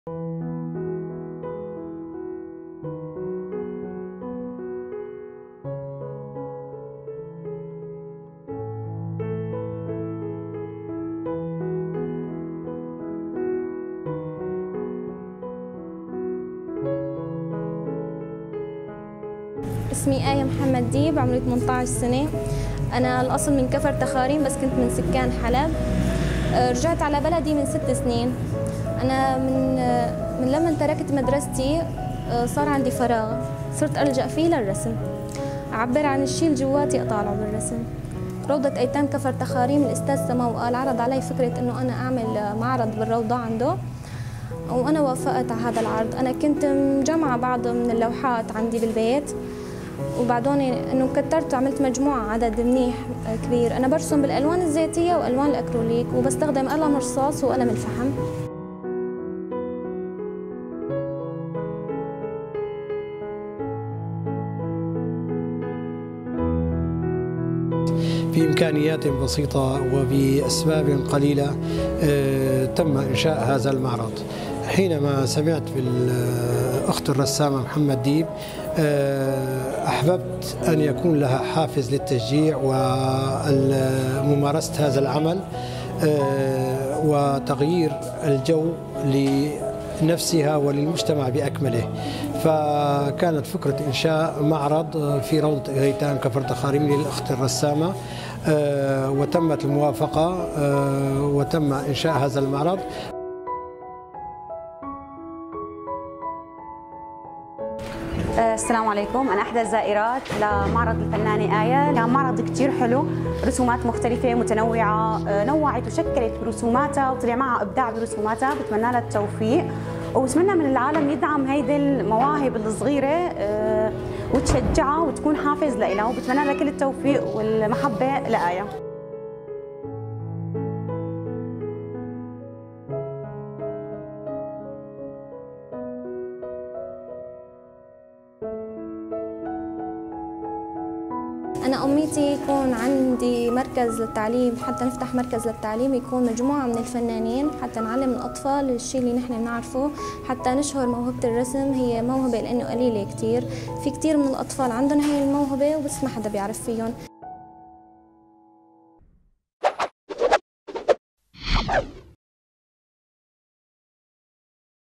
معرض للرسامة آية ديب في كفرتخاريم
أقامت الفنانة الشابة آية ديب معرضاً لرسوماتها في روضة أيتام كفرتخاريم